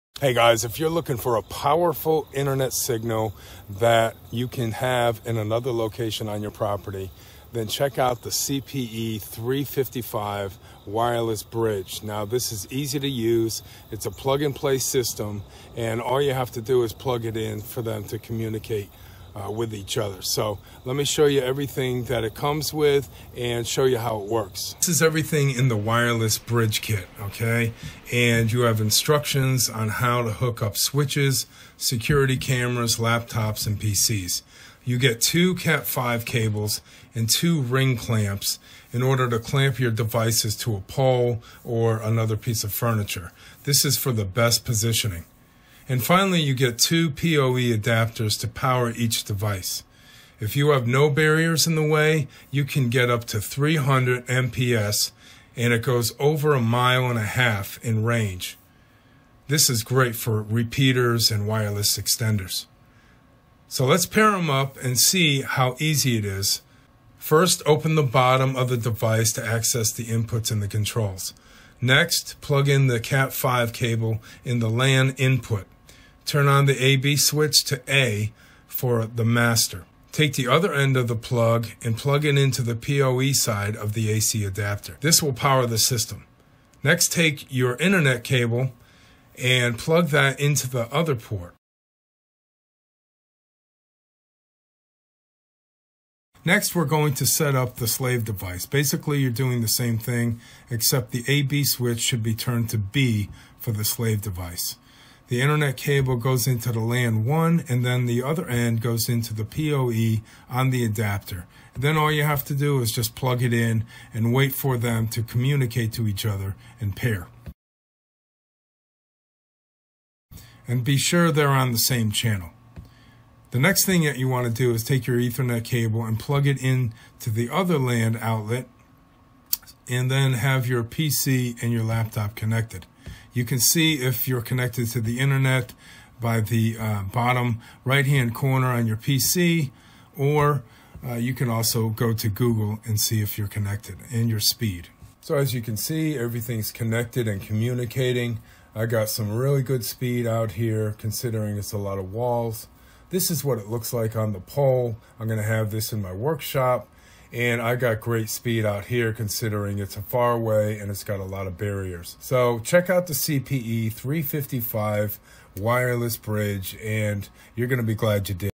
5.8GHz Outdoor Wireless Bridge Point to Point Access PTP/PTMP Plug & Play CPE Network Extender
The video to show you how to install the eoqo 5.8G wireless bridge, how to pair the both master and slave bridge together.

You can purchase this product on amazon, the url is